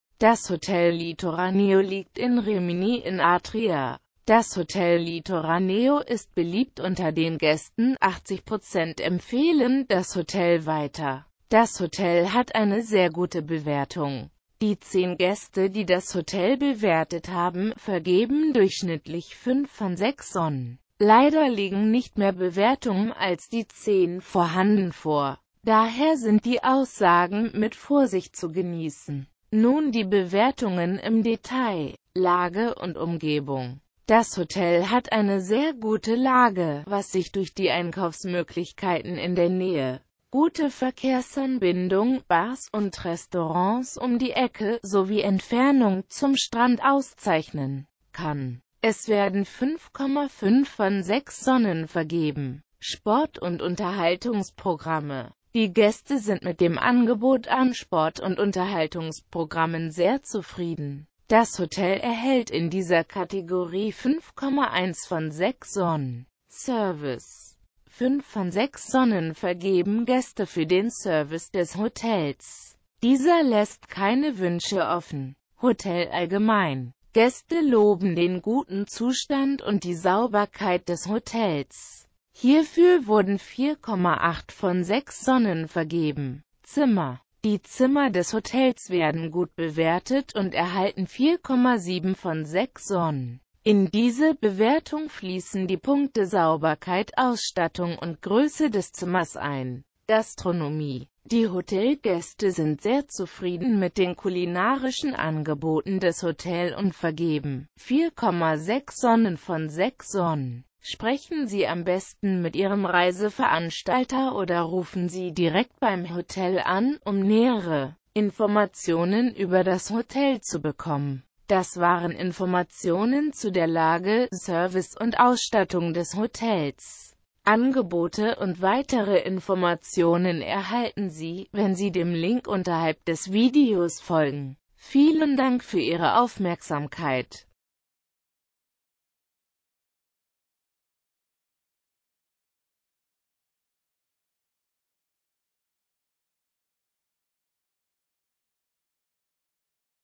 Hotel Litoraneo in Rimini (Adria - Italien) Bewertung
Das Hotel Litoraneo liegt in der Stadt Rimini welche zu Italien (Europa) gehoert.

 Das Hotel Litoraneo ist beliebt unter den Gaesten, 80% empfehlen das Hotel weiter. Zudem erhaelt das Hotel 5 von 6 Sonnen. Sprechen Sie am besten mit Ihrem Reiseveranstalter oder rufen Sie direkt beim Hotel an, um mehr Informationen ueber das Hotel zu bekommen.
Lage und Umgebung:
Sport und Unterhaltungsprogramme:
Service:
 5 von 6 Sonnen vergeben Gaeste fuer den Service des Hotels. Dieser laesst keine Wuensche offen.
Hotel allgemein:
 Gaeste loben den guten Zustand und die Sauberkeit des Hotels. Hierfuer wurden 4,8 von 6 Sonnen vergeben.

Informationen zur Verfuegbarkeit von Transfer zum Hotel, WLAN in den Zimmern des Hotel Litoraneo, sowie Details zum Animations-Progamm, Entfernung zum Strand oder zu Diskotheken erhalten Sie bei Ihren Reiseveranstalter oder auf der Webseite des Hotels. Neben den hier verwendeten Bewertungen, lohnt sich ein Blick auf die ggf. vorhandene Hotel Litoraneo Facebook-Seite.

Quellenhinweis:
Die verwendeten Informationen stammen von HolidayCheck.de. Sie wurden am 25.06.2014 abgerufen. In diesem Video wurden Bilder aus Rimini oder allgemeine Bilder aus Italien verwendet. Die gezeigten Orte und Landschaften dienen als Beispiel und muessen nicht unbedingt in der Naehe des Hotels liegen.